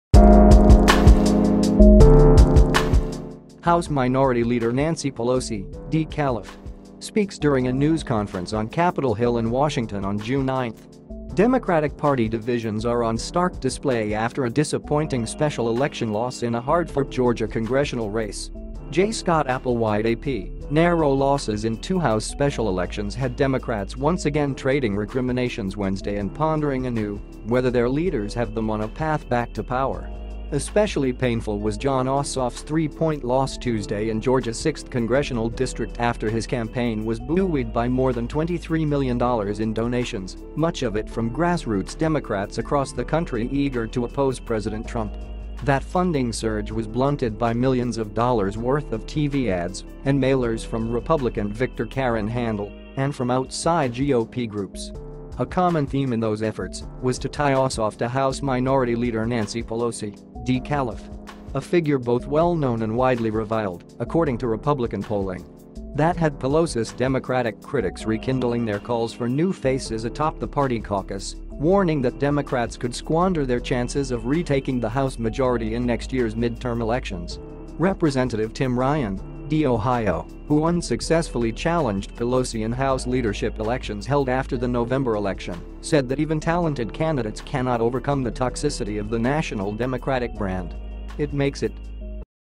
Democrats reel from another special election loss, and some point fingers at Pelosi
House Minority Leader Nancy Pelosi (D-Calif.) speaks during a news conference on Capitol Hill in Washington on June 9. Democratic Party divisions are on stark display after a disappointing special election loss in a hard-fought Georgia congressional race. (J. Scott Applewhite/AP)    Narrow losses in two House special elections had Democrats once again trading recriminations Wednesday and pondering anew whether their leaders have them on a path back to power. Especially painful was Jon Ossoffs three-point loss Tuesday in Georgias 6th Congressional District after his campaign was buoyed by more than $23 million in donations, much of it from grass-roots Democrats across the country eager to oppose President Trump. That funding surge was blunted by millions of dollars worth of TV ads and mailers from Republican victor Karen Handel and from outside GOP groups. A common theme in those efforts was to tie Ossoff to House Minority Leader Nancy Pelosi (D-Calif.)  a figure both well-known and widely reviled, according to Republican polling.  That had Pelosis Democratic critics rekindling their calls for new faces atop the party caucus, warning that Democrats could squander their chances of retaking the House majority in next years midterm elections. Rep. Tim Ryan (D-Ohio), who unsuccessfully challenged Pelosi in House leadership elections held after the November election, said that even talented candidates cannot overcome the toxicity of the national Democratic brand. It makes it